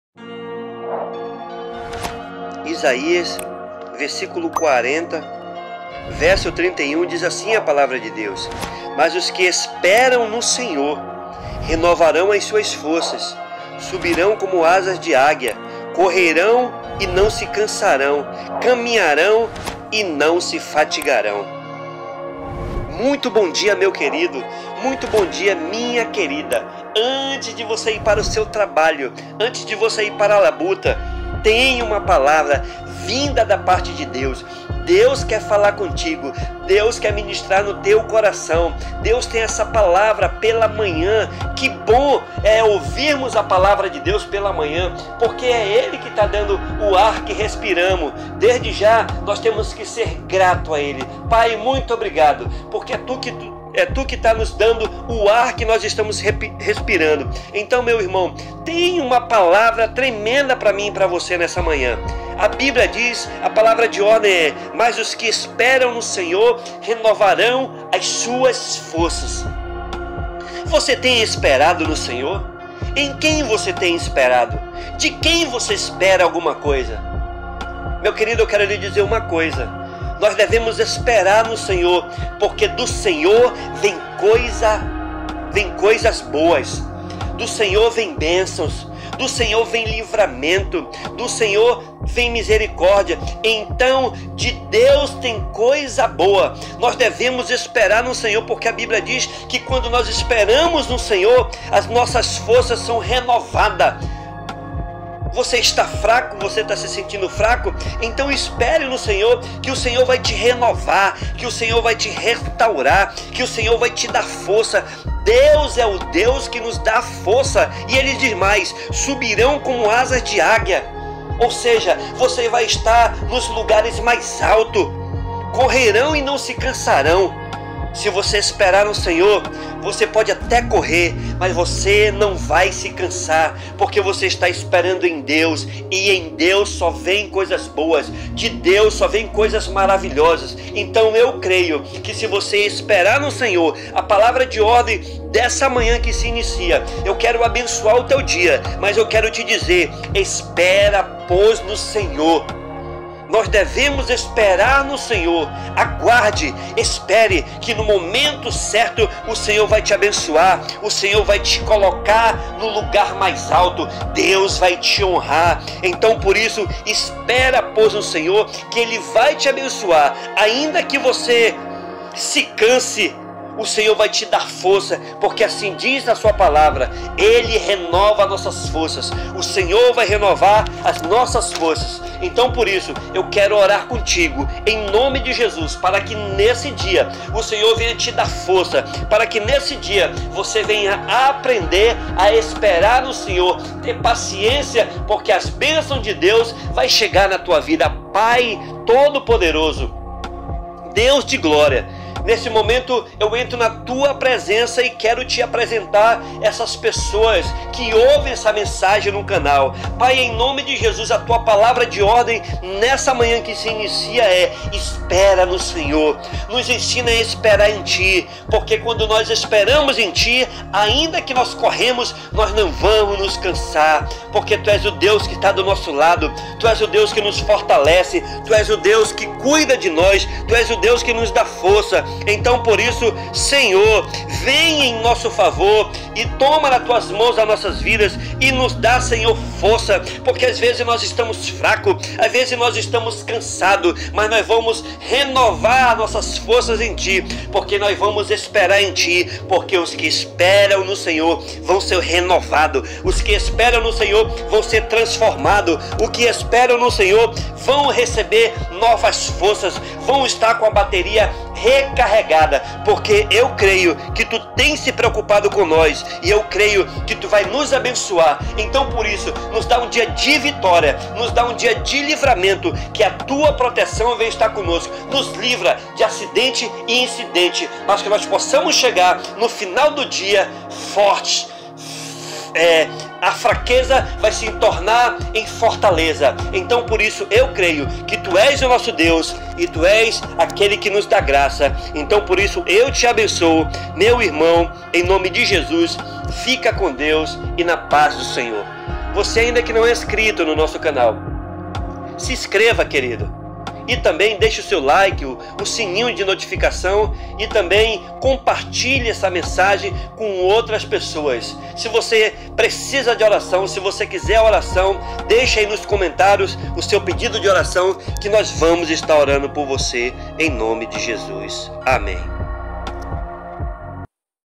Oração da Manhã...01.06...DEUS RENOVARA TUAS FORÇAS🙌
Feche os olhos respire fundo e sinta Deus falando com você através dessas orações, deixe o Espirito Santo invadir seu coração...Deus virá em seu socorro...sinta-se como você estivesse ouvindo Deus falar, abra seu coração pra Deus, só Ele lhe entenderá...nada esta perdido pra Deus...Ele é soberano. converse com Deus...ele te escutará